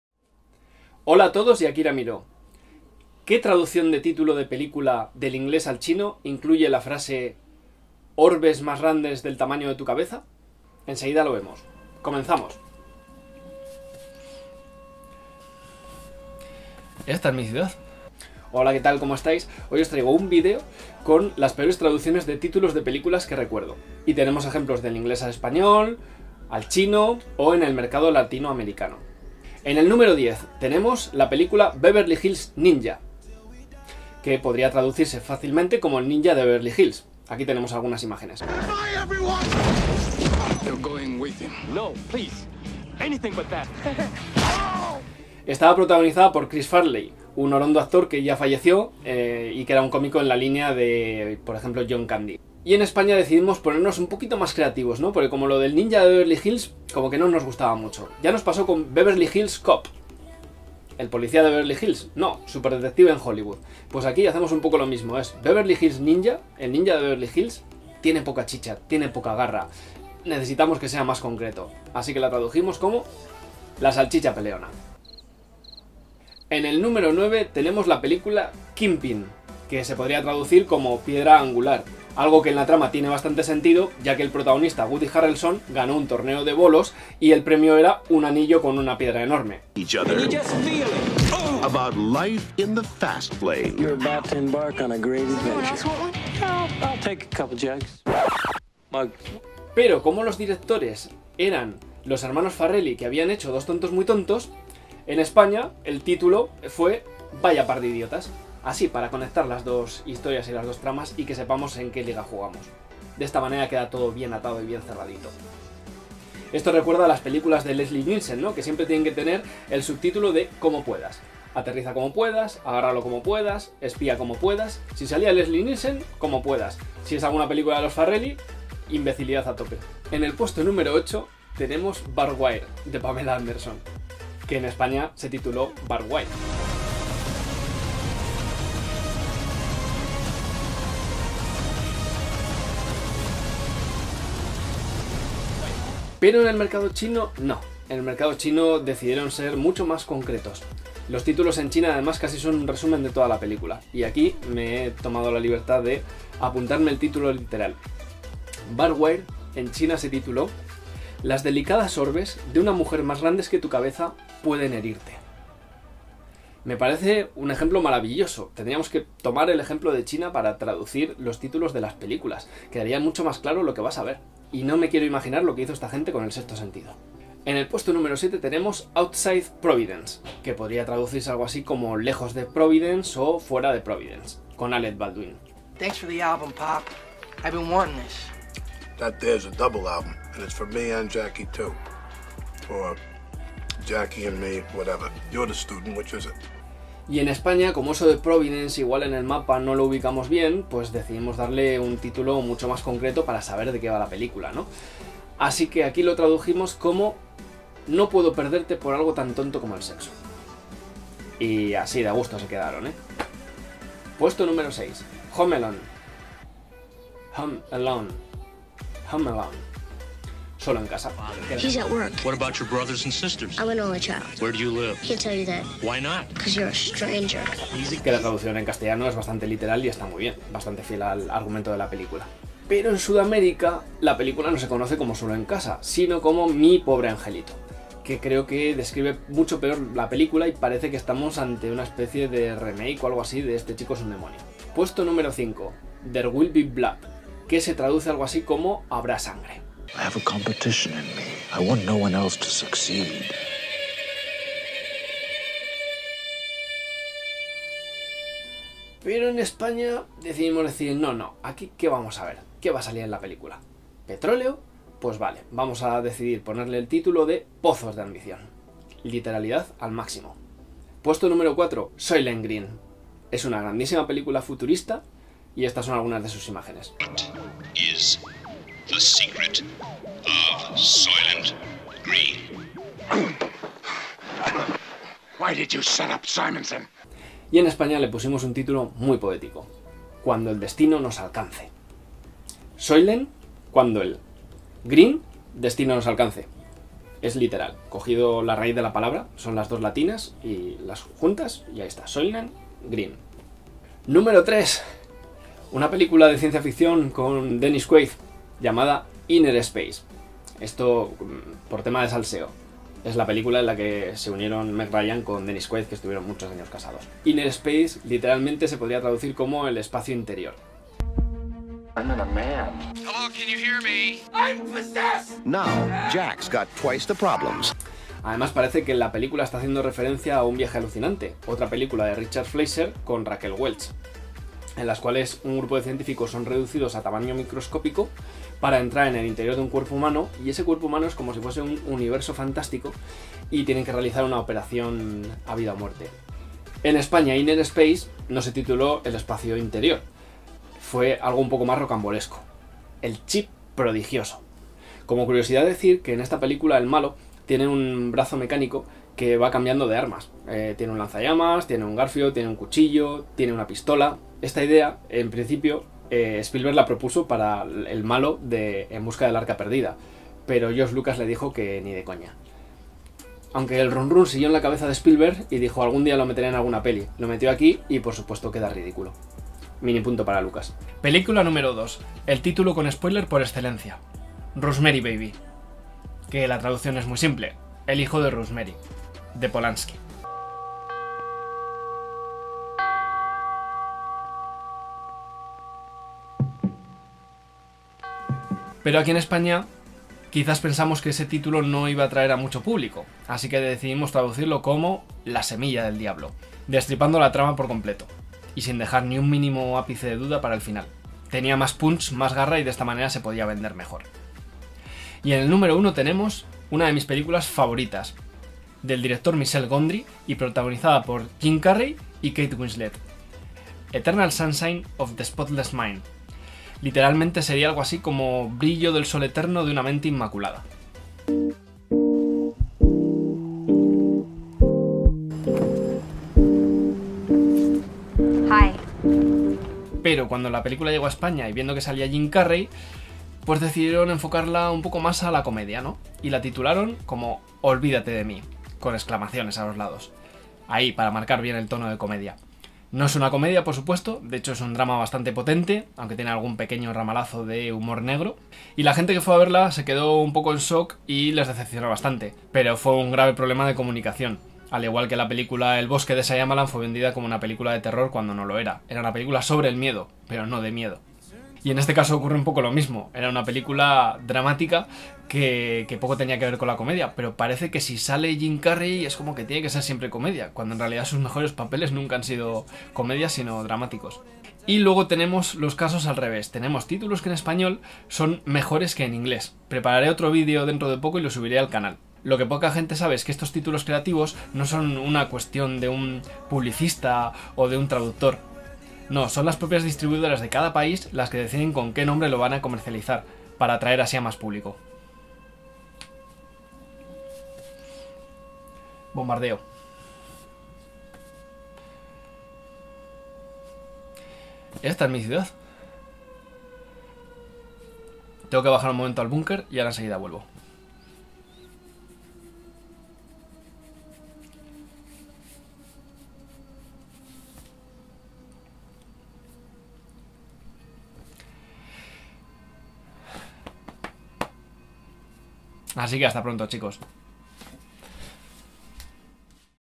Las peores traducciones de títulos de la historia (por O´Trevor)
Las distribuidoras a veces se ponen creativas y nos regalan algunos títulos de película inolvidables.